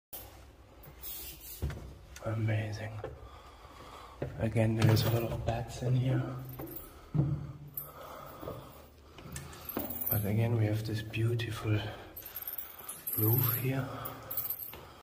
The inner Chamber of the Meidum Pyramid
This is the Inner Chamber of the Meidum Pyramid in Egypt.

It is located South of Giza and the Great Pyramid and the Sphinx.